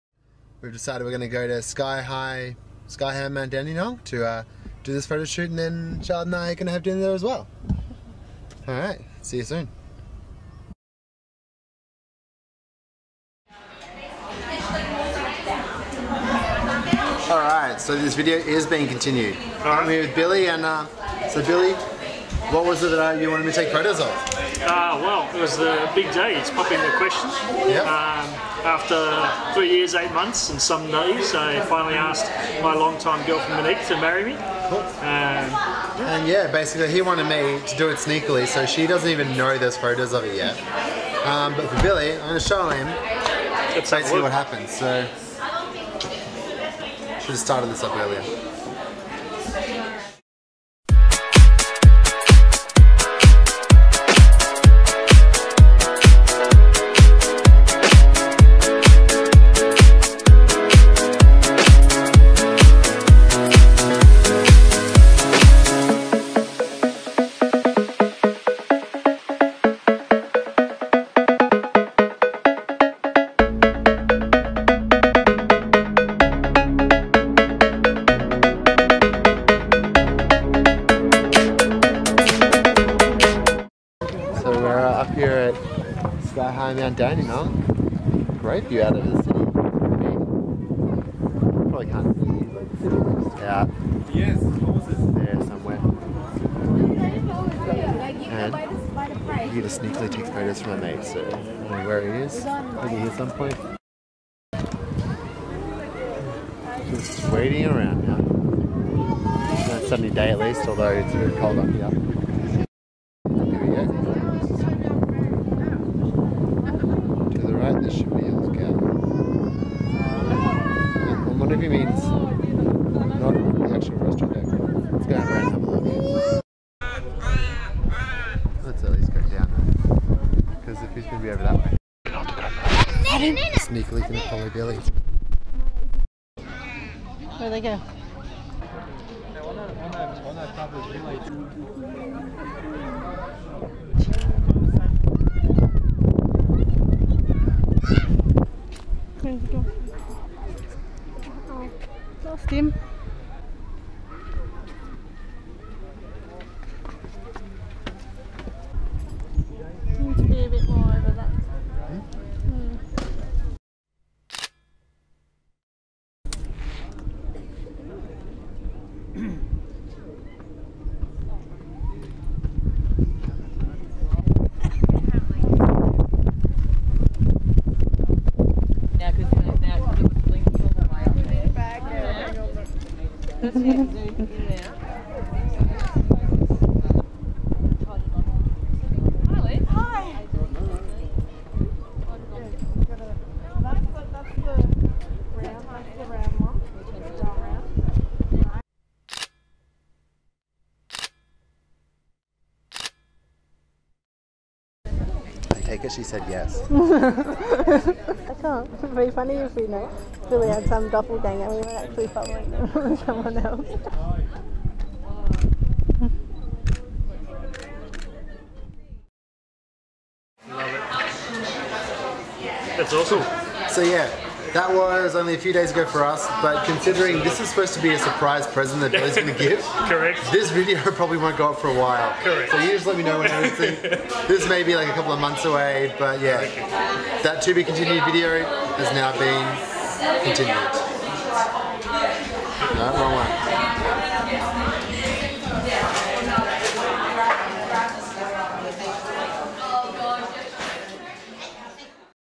Billy's Big Day
The video is actually continued! Find out more about the photo shoot I did for my mate that evening and why it was kept secret for a while.